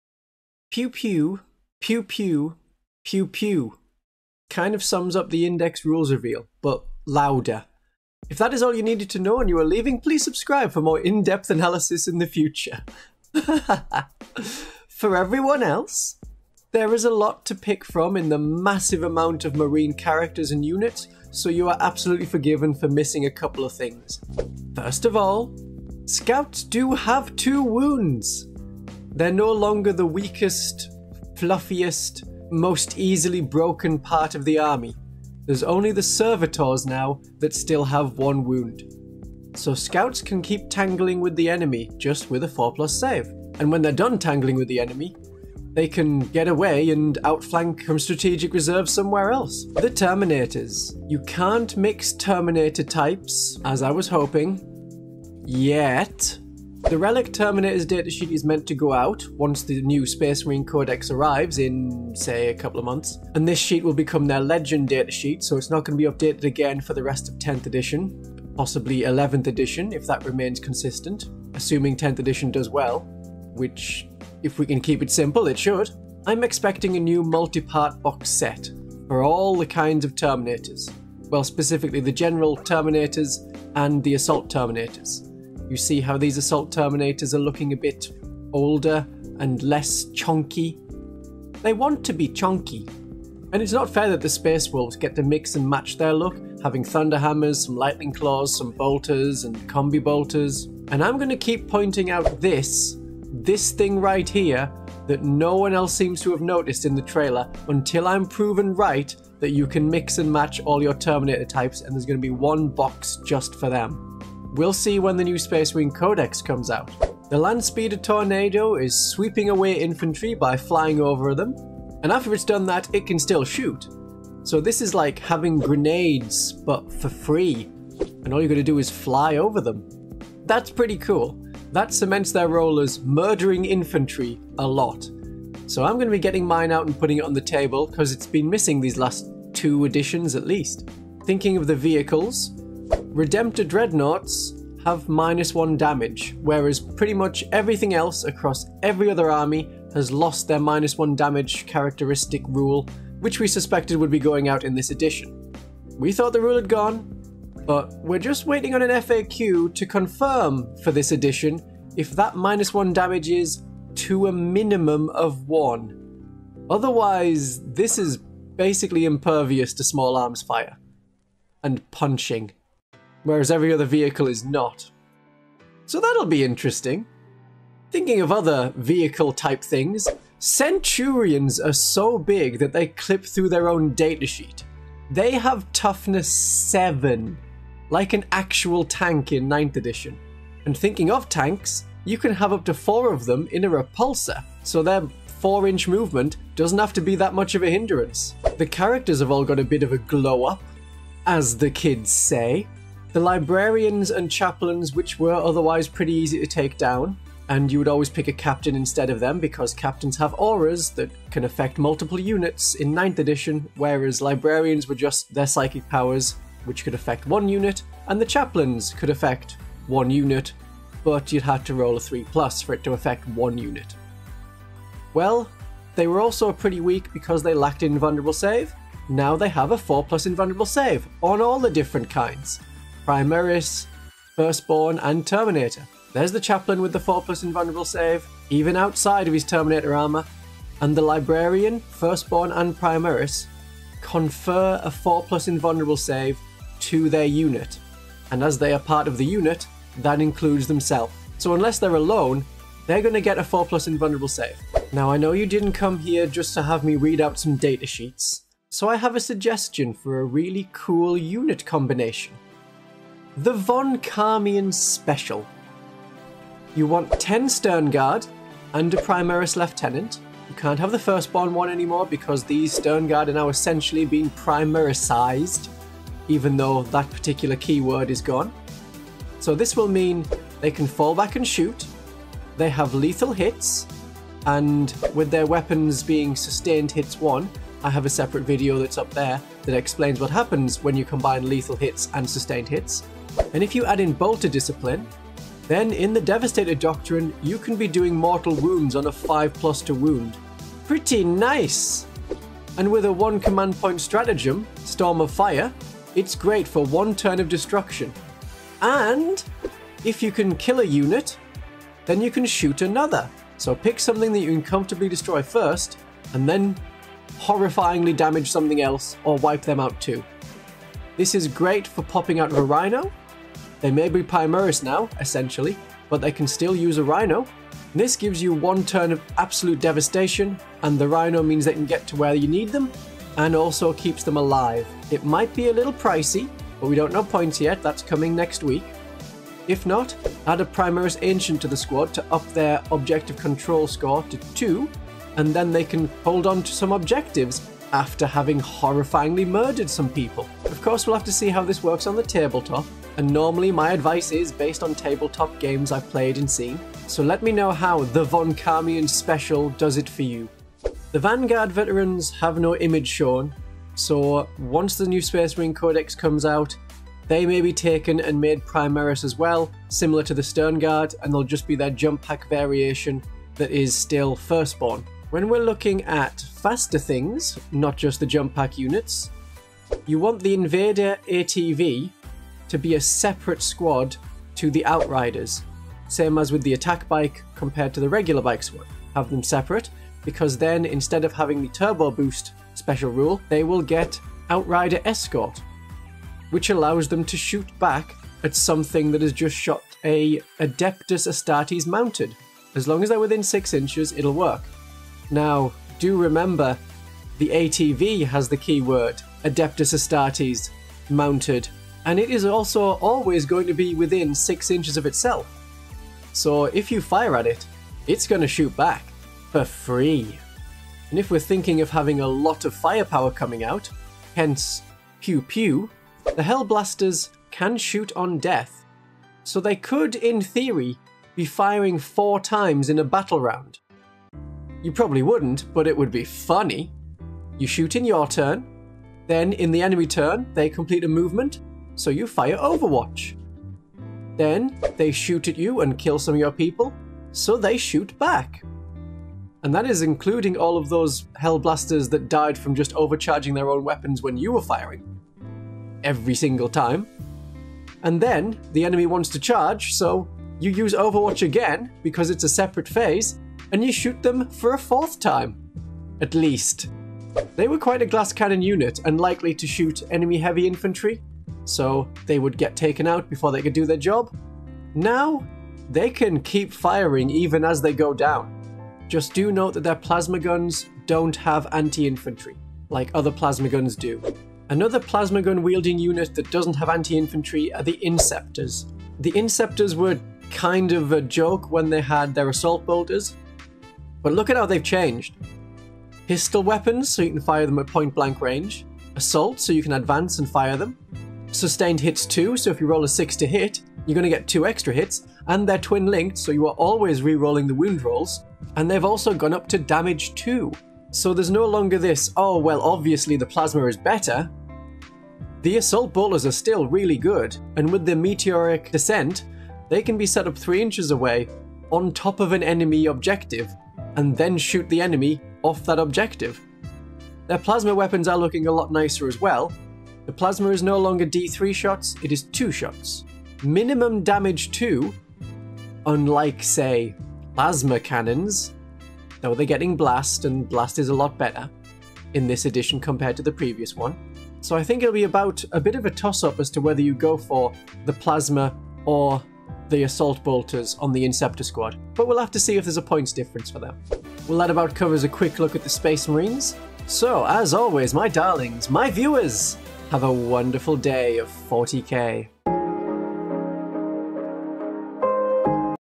2 Wound Scouts + MORE Things You MISSED: Space Marine Index Rules Reveal
I have a great Sternguard combo for massive damage in your games of Warhammer 40,000 as we look at the Space Marine Index Rules Reveal and uncover MORE things you may have missed.

We'll also be discussing other important rules and strategies that you need to know to dominate the battlefield. Whether you're a seasoned veteran or a new player, this video is packed with valuable insights and tips that will help you take your game to the next level. So, grab your index free from the Warhammer Community and join us as we explore the Space Marine Index Rules Reveal!

00:00 Pew pew
00:24 Scouts
00:49 New Terminator boxed set
02:08 Land Speeder Tornado
02:22 Redemptor Dreadnought
03:00 Centurians
03:18 Librarians and Chaplains
04:14 The Von Carmian Special (Sternguard)
06:00 Vanguard Veterans
06:19 Invader ATV and Attack Bike with Outrider squad
07:06 Hellblasters
08:04 Inceptors